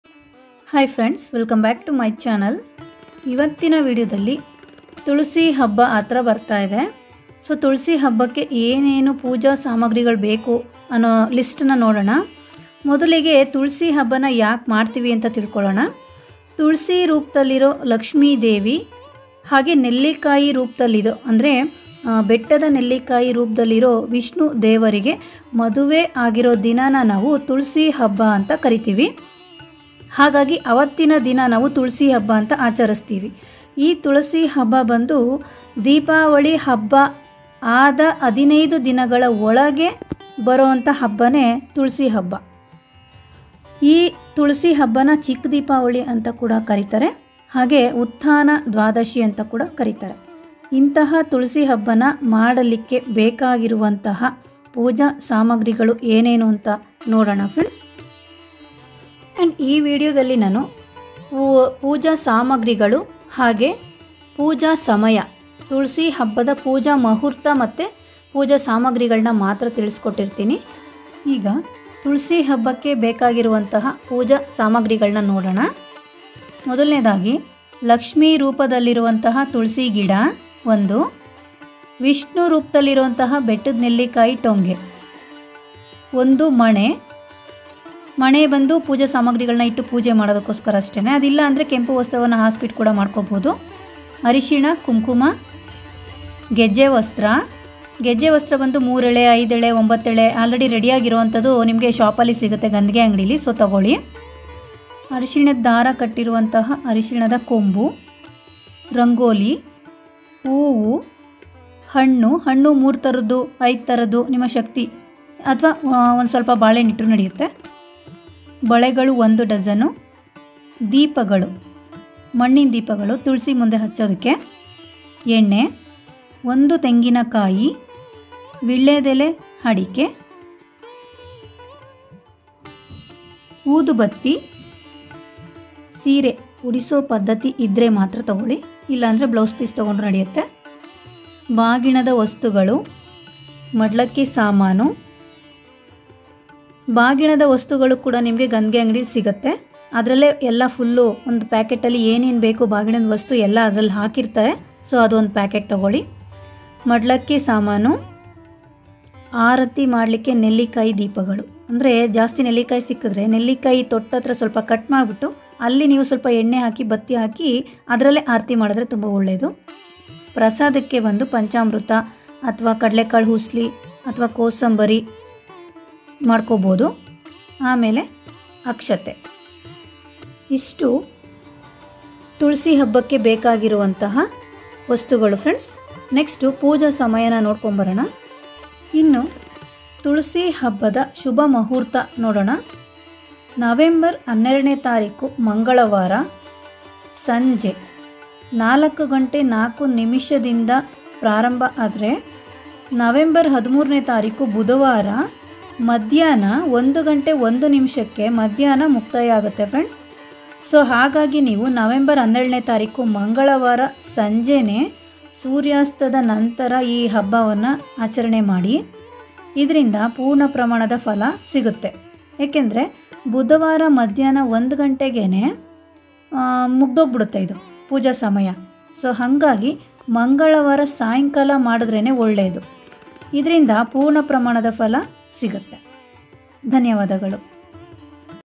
ತುಳಸೀ ಹಬ್ಬದ ಸರಿಯಾದ ಸಮಯ ಹಾಗೂ ಪೂಜಾ ಸಾಮಾಗ್ರಿಗಳ ಪಟ್ಟಿ tulasifestival Gaanacreations-vlogs
ತುಳಸೀ ಹಬ್ಬದ ಸರಿಯಾದ ಸಮಯ ಹಾಗೂ ಪೂಜಾ ಸಾಮಾಗ್ರಿಗಳ ಪಟ್ಟಿ